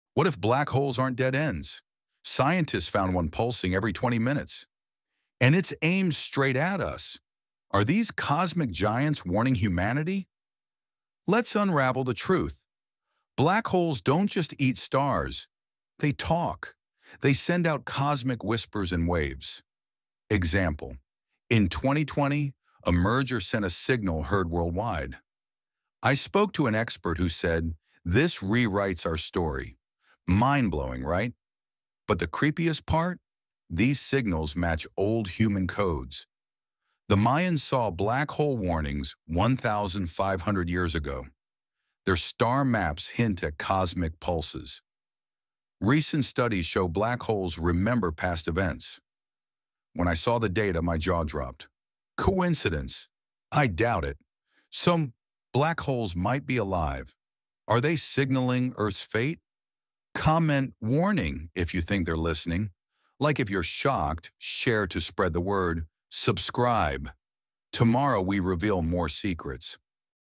Black Holes Are Alive – And They’re Sending Us Warnings
🌌 Did science get it ALL WRONG?

What if black holes aren’t silent voids but living sentinels with a message? The Cosmic Sentinel dares to question the norm and reveal truths mainstream science might fear.

⚛️ 3 Experiments That Will Unravel Reality

Peek into the unknown with experiments that stunned the world: one even Einstein couldn’t explain, lab results that twist logic, and a discovery buried in silence. The full story? You’ll have to watch to believe it.

🔥 WARNING: Shocking Secrets Revealed

Is reality an illusion shaped by black holes? Why have governments possibly hushed this up? What they don’t want you to know could rewrite our cosmic destiny. Think The Matrix meets Stranger Things—this is no ordinary tale.

🚨 Spoiler Warning: Turn back if your mind can’t handle the truth… or laugh at the thought!

🕒 Act Fast: The universe’s warnings are slipping out—don’t miss your chance to uncover them!

🌠 What’s Next?

Next video: This Galaxy Shouldn’t Exist – And It’s Pointing Directly at Earth ?